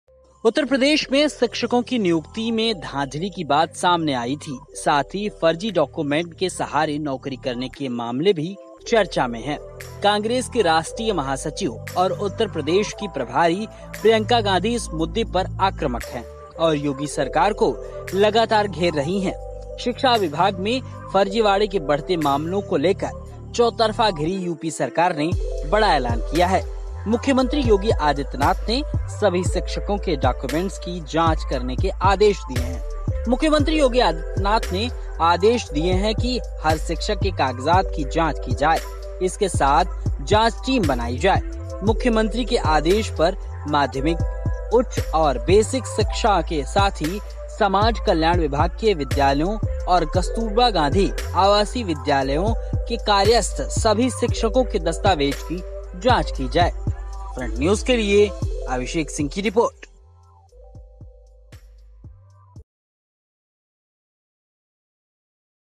Uttar Pradesh Teachers Recruitment Scam
उत्तर प्रदेश में शिक्षकों की नियुक्ति में धांधली की बात सामने आई थी. साथ ही फर्जी डॉक्यूमेंट के सहारे नौकरी करने के मामले भी चर्चा में हैं. कांग्रेस की राष्ट्रीय महासचिव और उत्तर प्रदेश की प्रभारी प्रियंका गांधी इस मुद्दे पर आक्रामक हैं और योगी सरकार को लगातार घेर रही हैं. शिक्षा विभाग में फर्जीवाड़े के बढ़ते मामलों को लेकर चौतरफा घिरी यूपी सरकार ने बड़ा ऐलान किया है.